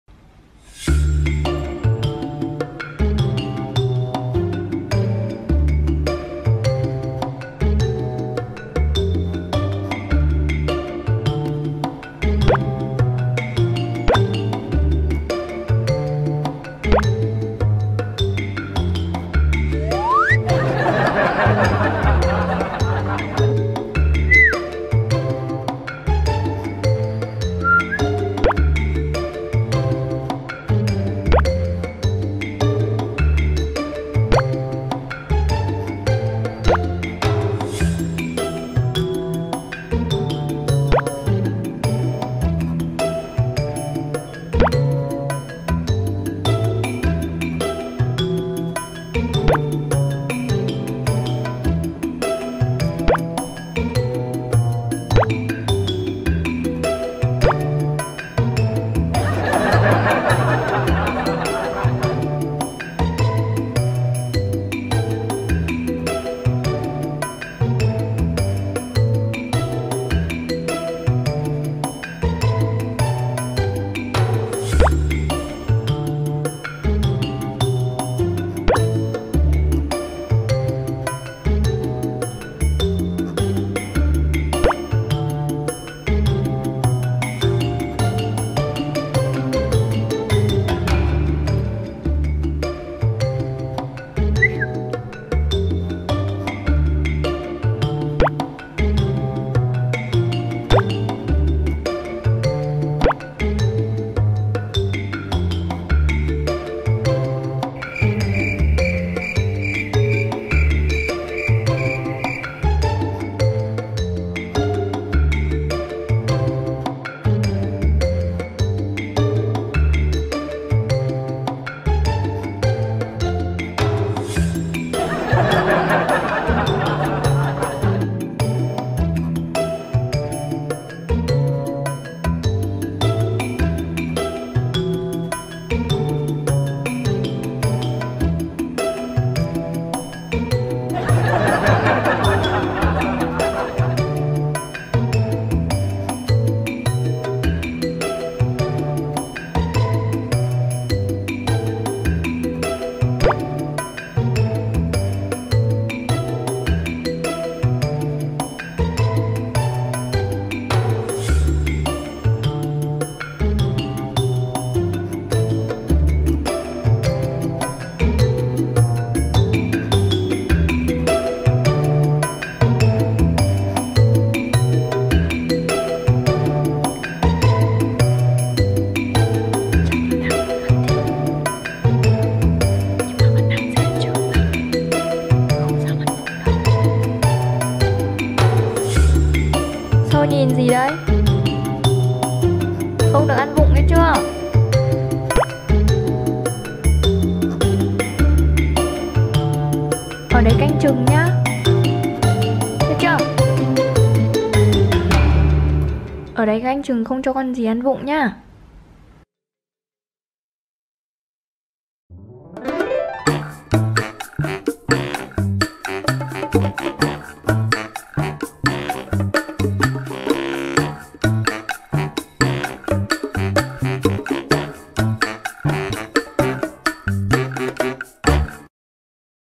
[Cô Kim] Mèo Sâu ngồi canh mâm chống trộm lấy trộm thịt của chủ nè
VÌ là mèo ăn chay nên cũng nhờ được việc canh mâm nhé. Không chỉ có chõ mới làm được việc í đâu nha.